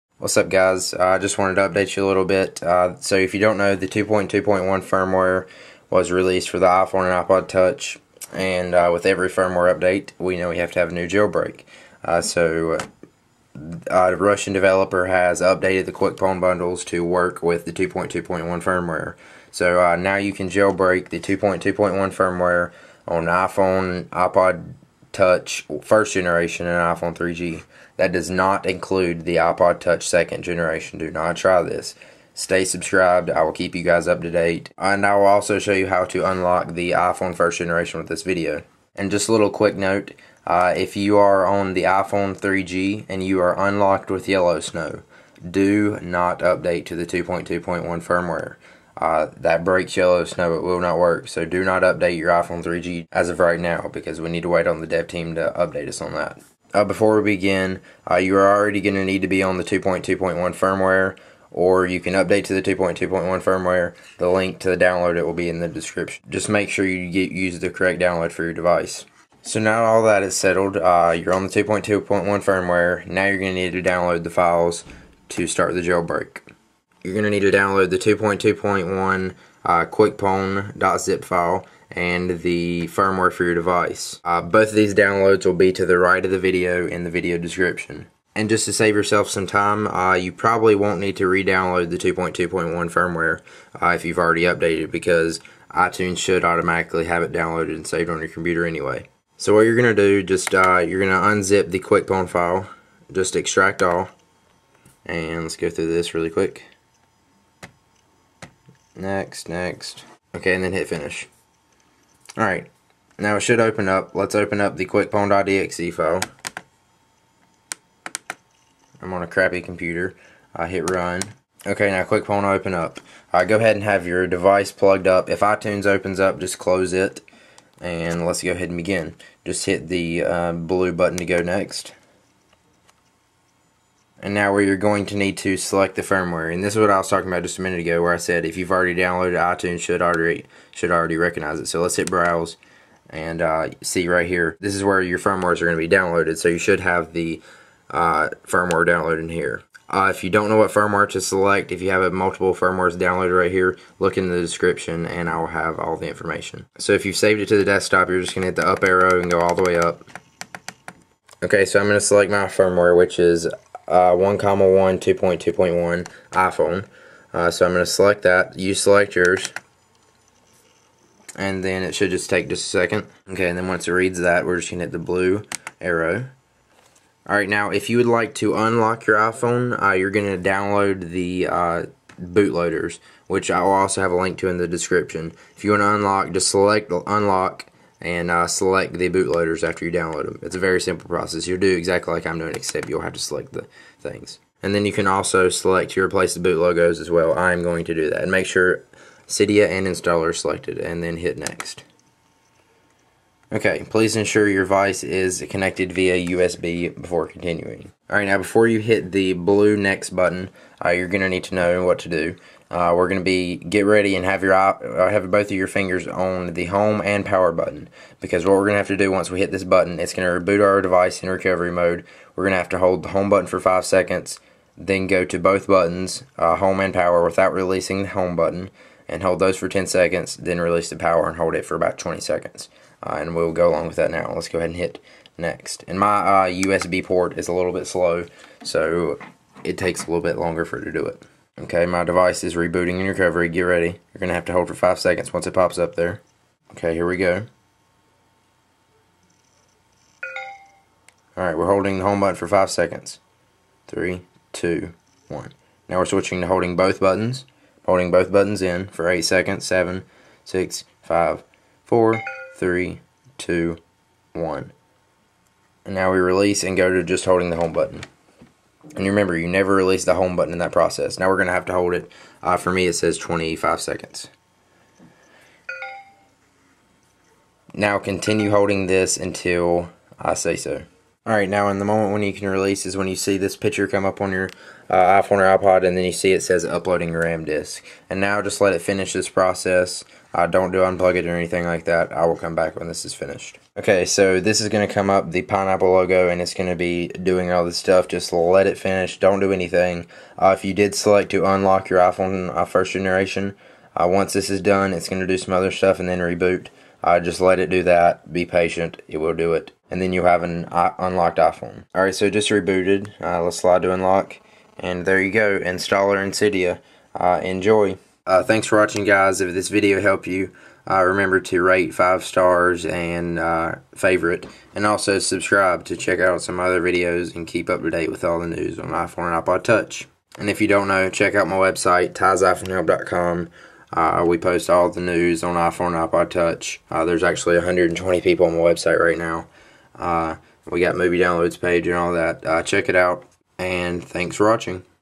Jailbreak & Unlock 2.2.1 iPhone 2g, 3g, and iPod Touch
Notes:
-Your device must already be on the 2.2.1 firmware.
-If you have an iPhone 3g and it is unlocked with Yellowsn0w DO NOT update to the newest firmware
-You must have the .net framework installed on your windows computer
-If you are running Macosx 10.5.6 you HAVE to read the quick guide posted by the dev team so your computer will read DFU mode. Otherwise it will mess up and you will have to restore your device

In case you don't know, here are the firmware names for the devices
iPod Touch 1g - iPod1,1_2.2.1_5H11_Restore.ipsw
iPhone 2g - iPhone1,1_2.2.1_5H11_Restore.ipsw
iPhone 3g - iPhone1,2_2.2.1_5H11_Restore.ipsw